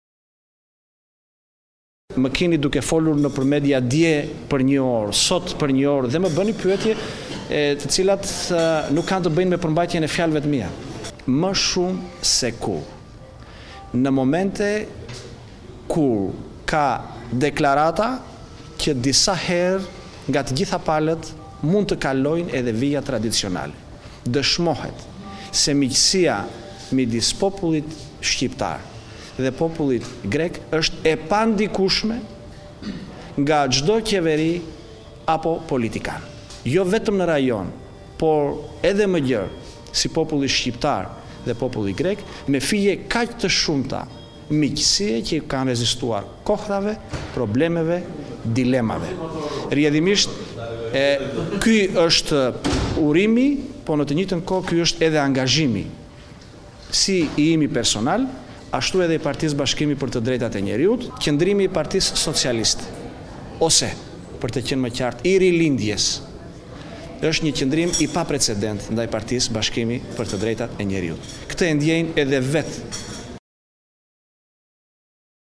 Report TV - Deklarata e deputetes së Qipros Dule:S'ndikon në raportet me Greqinë
Kryetari i Partisë Bashkimi për të drejtat e njeriut nuk ka komentuar drejtpërdretejt deklaratat e një dite më parë të deputetes greke të Qipros, Eleni Theoharous e cila tha se Vorio Epiri është në Tiranë.

Por Dule tha se deklarata të politikaneve të caktuar nuk ndikojnë në marrëdhëniet miqësore mes popujve shqiptarë dhe grekë.

Kreu I PBDNJ-së tha gjithashtu se forca e tij politike kohët e fundit është përballur me sulme të paprecedent nga kreu i PS-së, ndërsa përjashtoi një koalicion të mundshëm me këtë forcë politike.